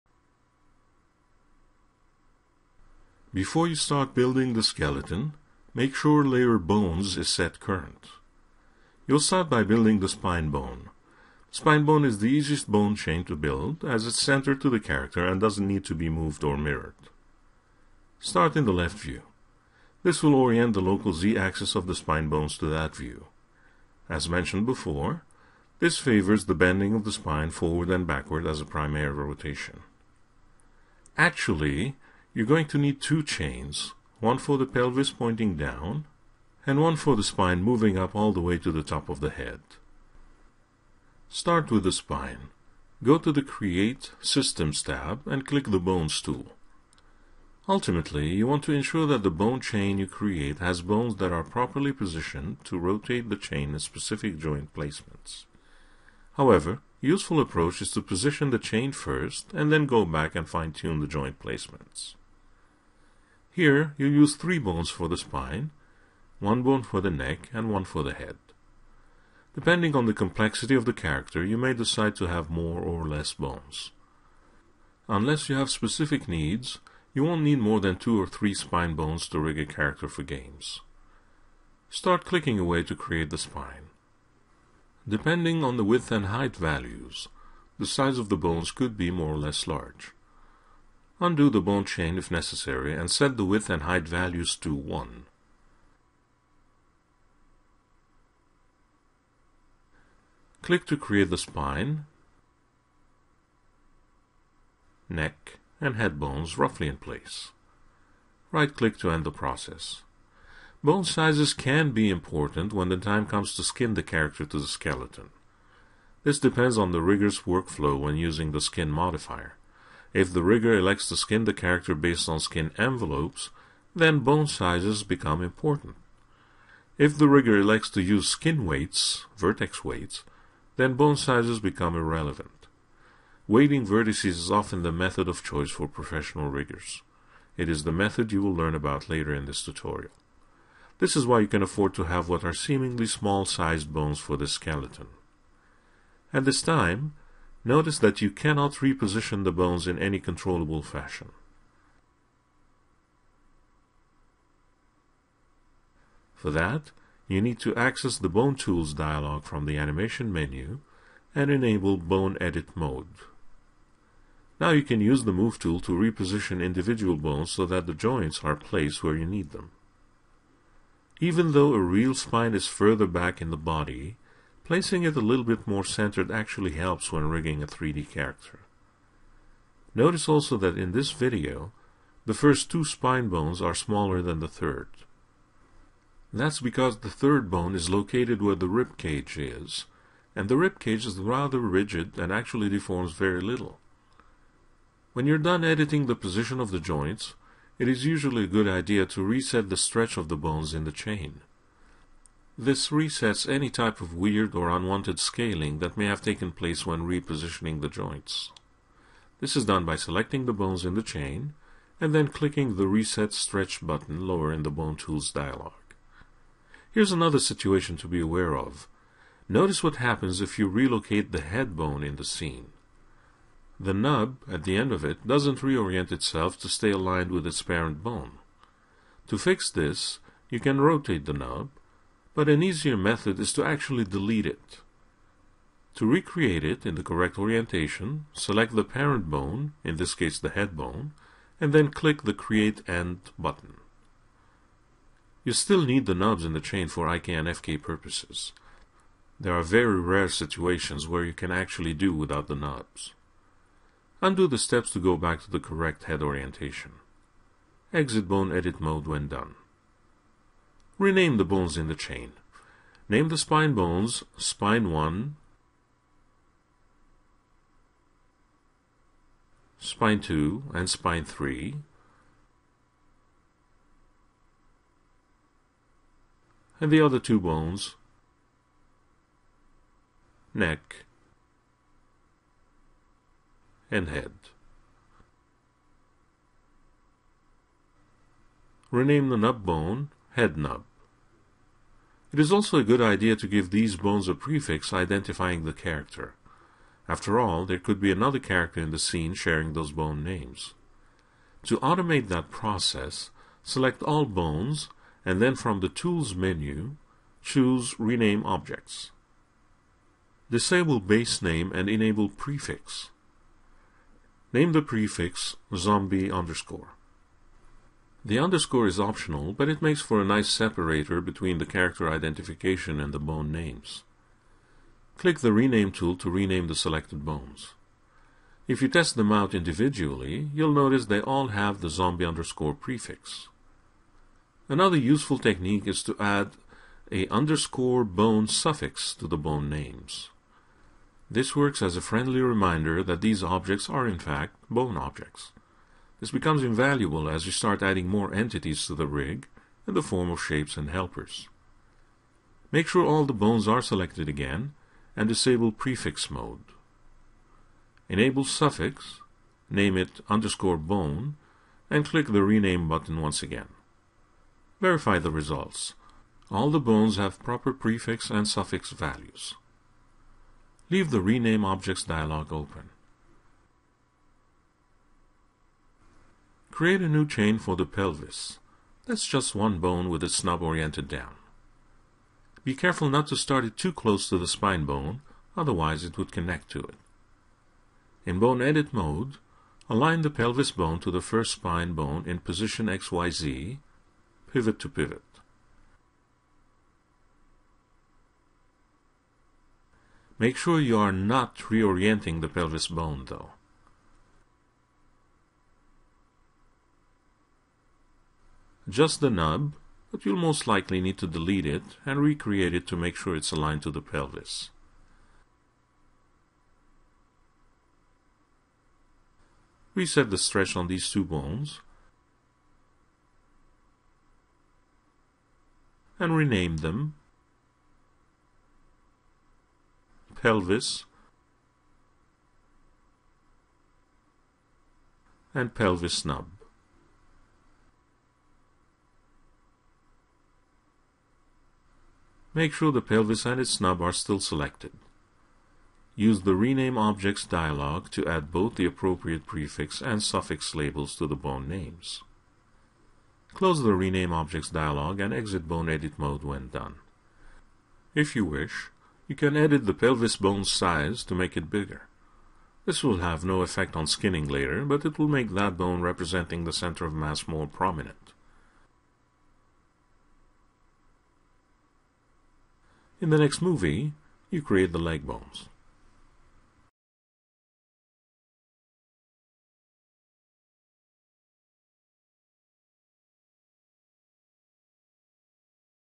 Creating a Skeleton in 3ds Max - Part 1 - Spine Bones
In this 3ds Max multi-part series, you learn to build a bone skeleton for your character. This process is only about building the skeleton. You will cover rigging, constraints, and IK solvers at a later time. You start building the spine chain as it is the easiest bone chain to create and place.
Level: Advanced
Recorded in: 3ds Max 2013